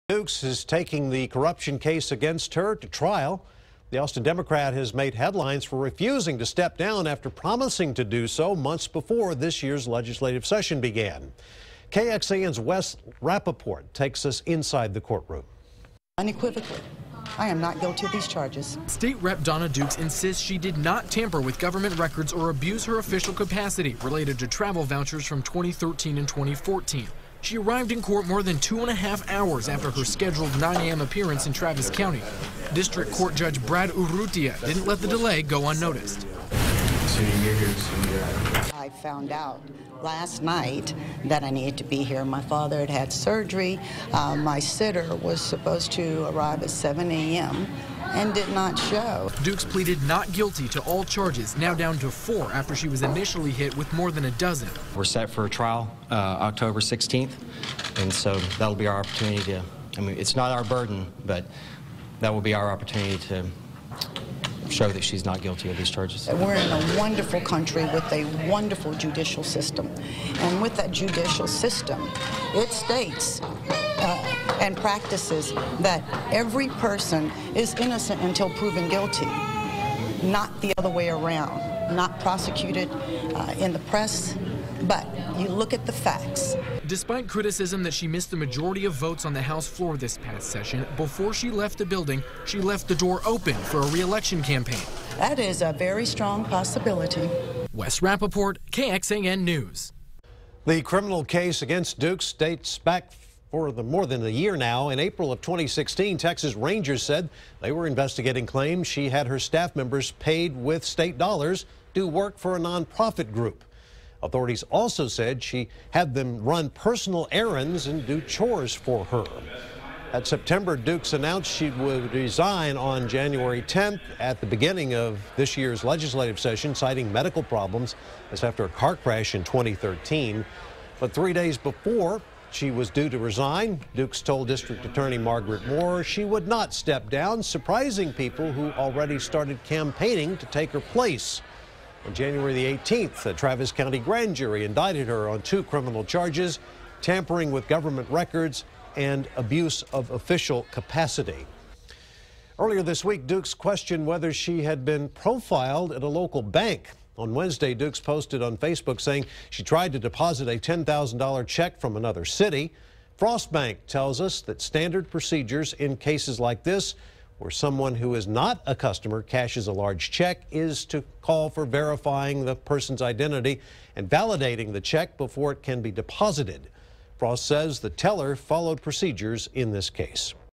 Rep. Dawnna Dukes shows up in court, trial date scheduled
Rep. Dukes showed up to court more than two hours after her 9 a.m. scheduled start time.